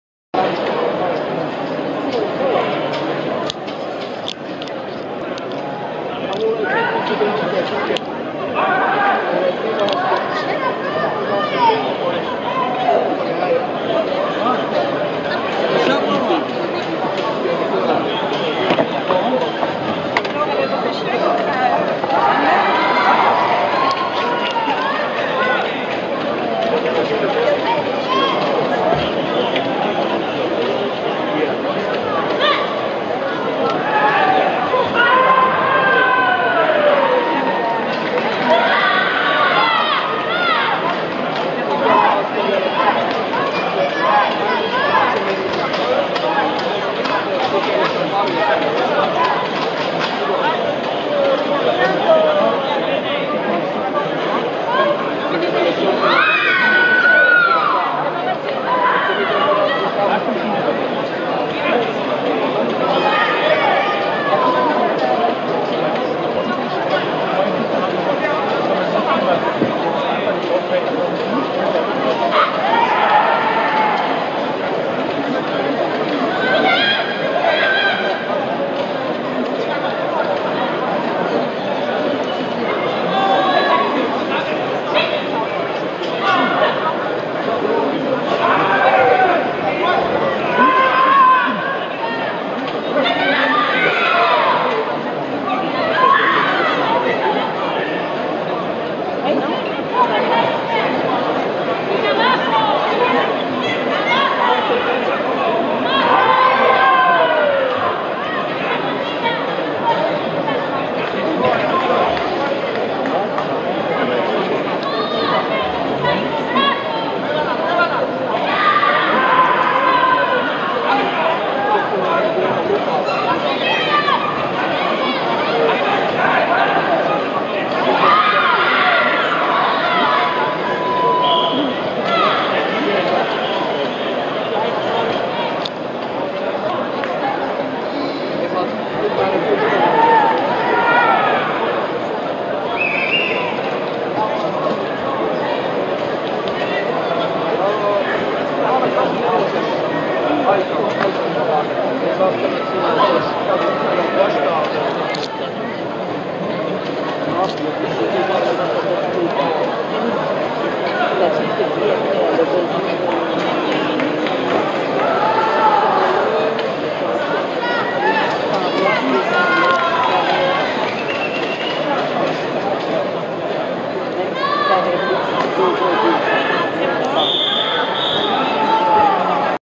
World Championships 2016, kumite female over 68kg, Aco Isabela PER vs tatarova SVK
kumite female over 68kg, pool 1, round 2

aka Aco Isabela Mallory PER
ao Tatarová Dominika SVK

winner by flags ao Tatarová